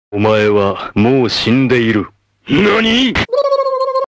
omae wa mou shindeiru but it has an anti climatic sound at the end
after one week of thinking of something funny, this was the only thing that Jaren could think of.